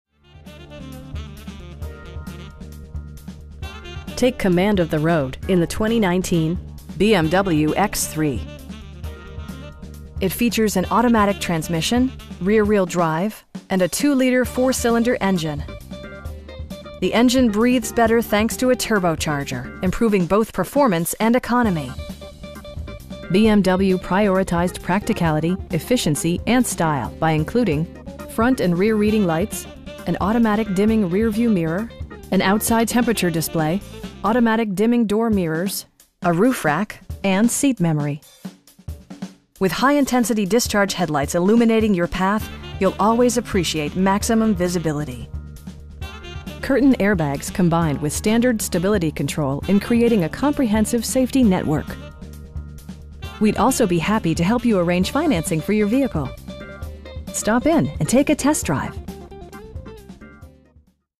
2019 BMW X3 sDrive30i in Ocala, FL 34474
BMW of Ocala
5145 SW College Rd

Take command of the road in the 2019 BMW X3. It features an automatic transmission, rear-wheel drive, and a 2 liter 4 cylinder engine. The engine breathes better thanks to a turbocharger, improving both performance and economy. BMW prioritized practicality, efficiency, and style by including: front and rear reading lights, an automatic dimming rear-view mirror, an outside temperature display, automatic dimming door mirrors, telescoping steering wheel, a roof rack, and seat memory. With high intensity discharge headlights illuminating your path, you'll always appreciate maximum visibility. Curtain airbags combine with standard stability control in creating a comprehensive safety network. We'd also be happy to help you arrange financing for your vehicle. Stop in and take a test drive!